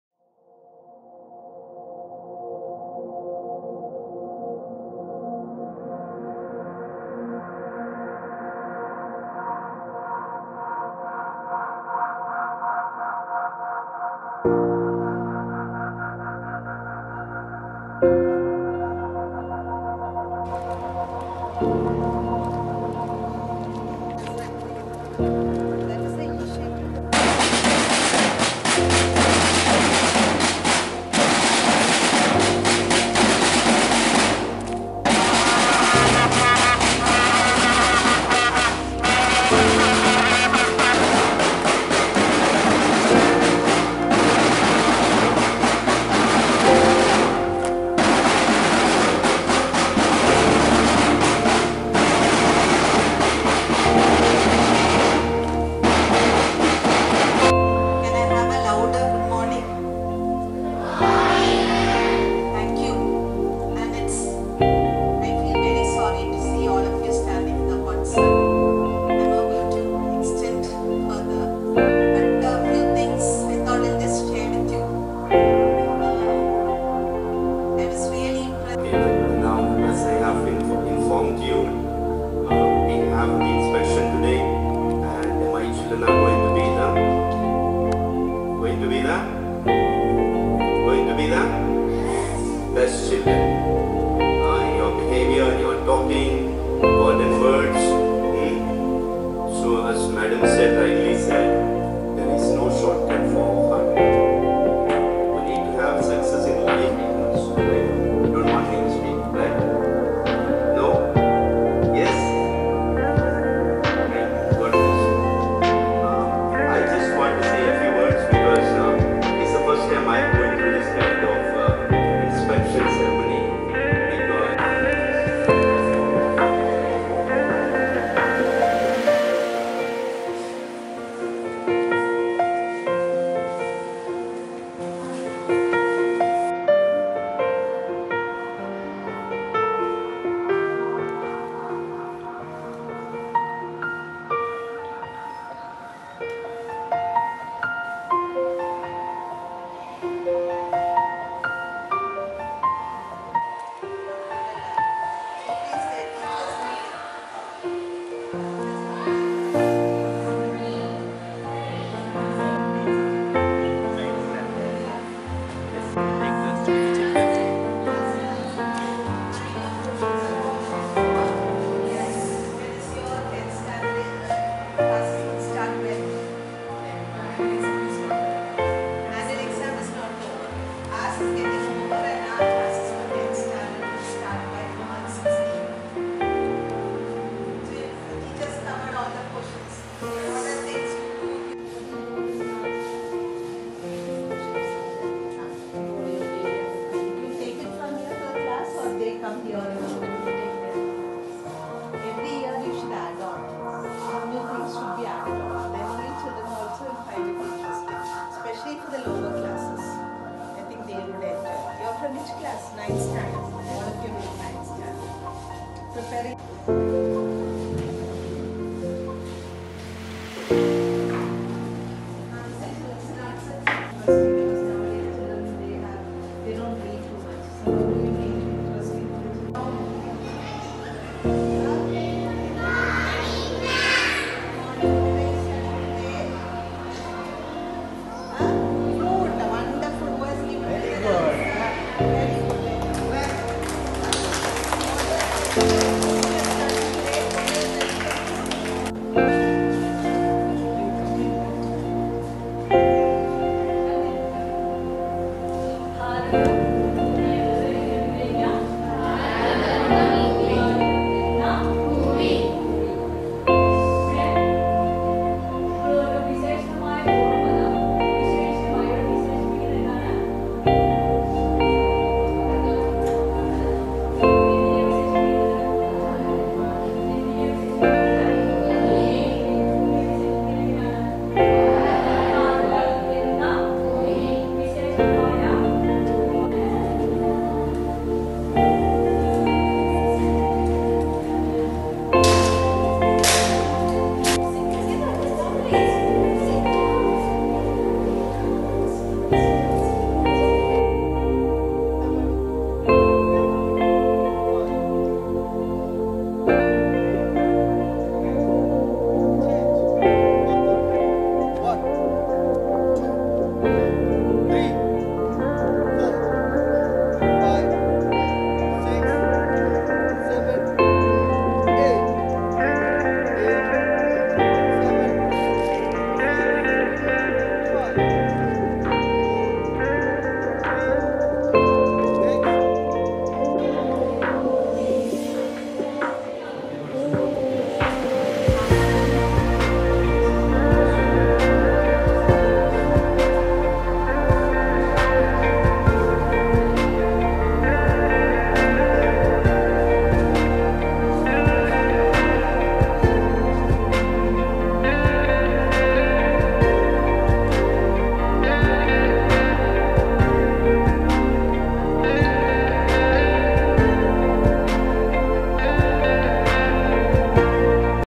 CBSC INSPECTION, 25/02/2020
Description of HOLY CROSS SCHOOL AYMANAM on inspection day